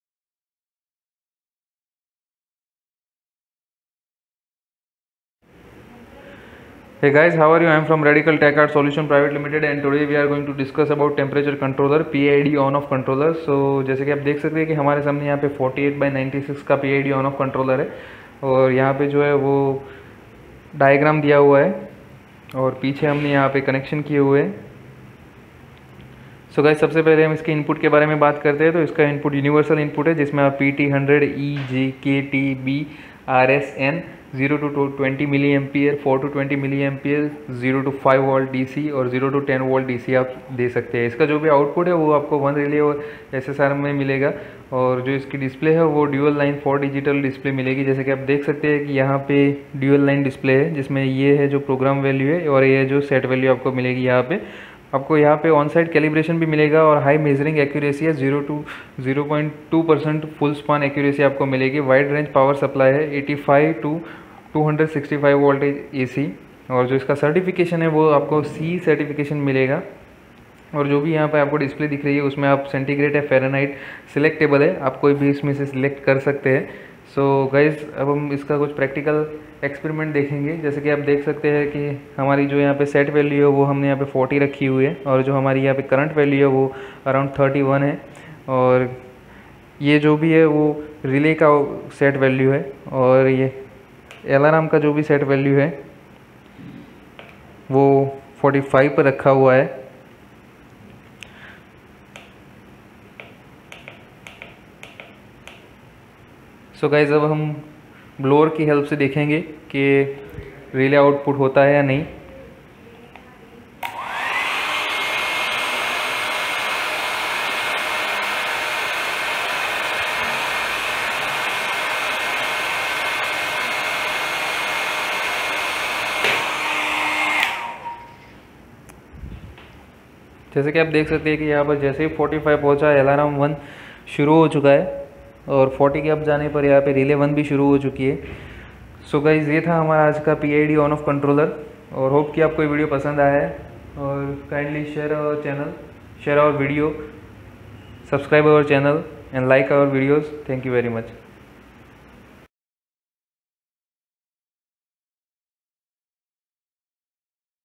Temperature (PID/On-Off Controller) Size : 48*96
A Temperature Controller is an essential device used to maintain a set temperature with high accuracy and stability. Whether you're working in a laboratory, industrial setting, or home automation, understanding PID and On-Off Controllers is crucial.

Size : 48*96mm
Input : Universal (PT-100, E, J, K, T, B, R, S, N , 0-20 mA, 4-20 mA, 0-5 VDC, 0-10 VDC)
Output: 1 Relay/ SSR , 1 Relay
Display: Dual Line 4 Digit Display (Red for Process Value and Green For Set Value)
On site Calibration Facility
High Measuring Accuracy (0.2% of Full Span)
Wide Range of Power Supply: 85-265 VAC
Housing : Black ABS, Fire Retraded
Certification : CE Certified
Display : Deg C and Deg F Selectable
SV Setting higher and Lower Limit Configurable
Quality assurance:
1. 12 months warranty
2. Our defective ratio is less than 0.3%